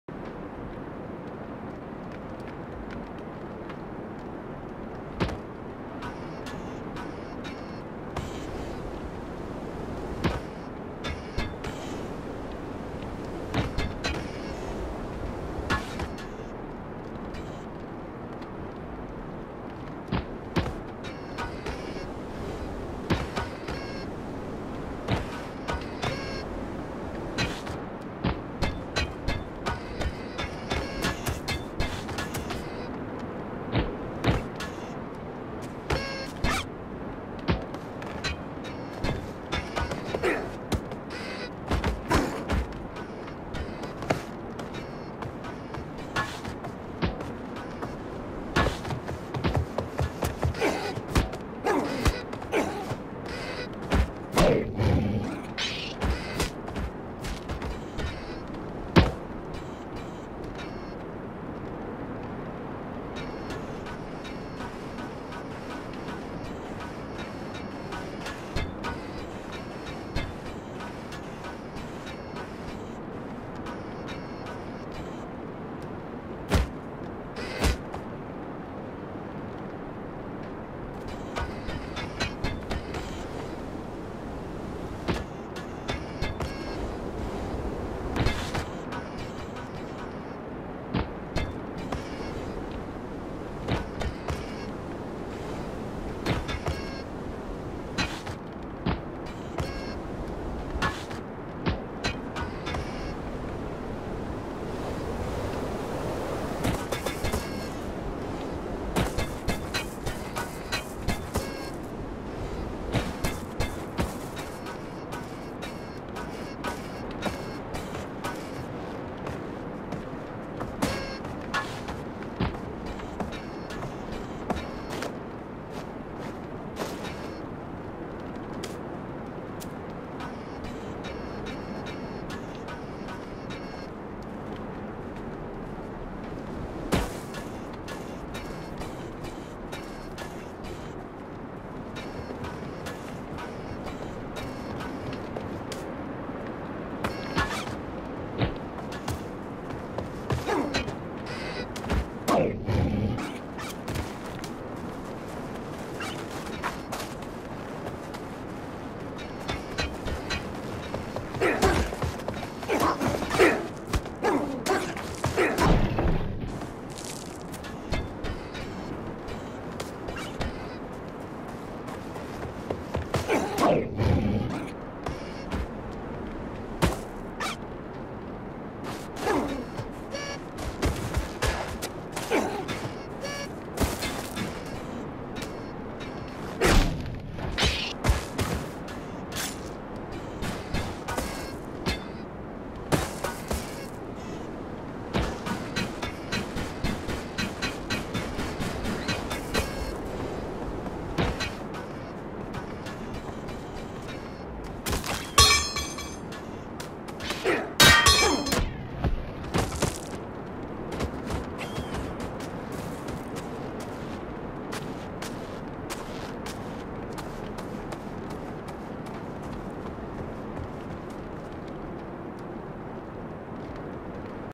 Bastion trys to destroy a small camp of bipedal rabbits
this is just me dicking around with recording software, mods for the game, and a really great map to test with..

Map: BoneHead by: Roy W Winchester
Bastion model by:Akazi